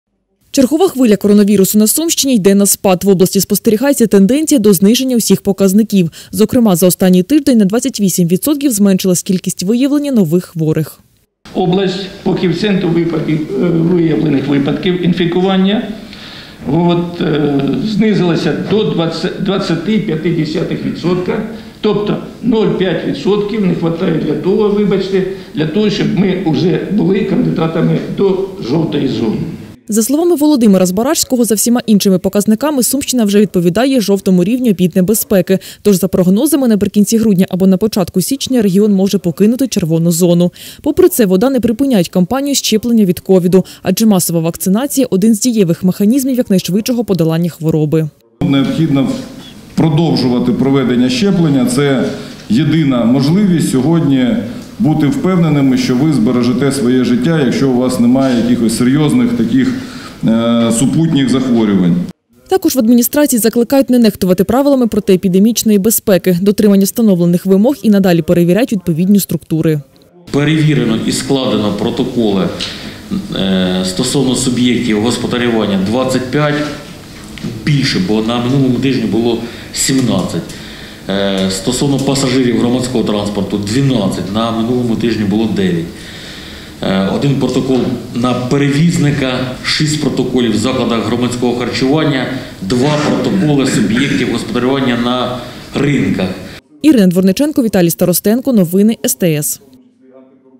Сумщина незабаром може перейти до «жовтої» карантинної зони
Протягом останніх тижнів коронавірус на Сумщині вщухає. За минулі 7 днів в області зменшились показники виявлення нових хворих та рівень госпіталізації. Тож, ймовірно, вже незабаром регіон перейде до «жовтої» карантинної зони. Усі подробиці – дивіться далі.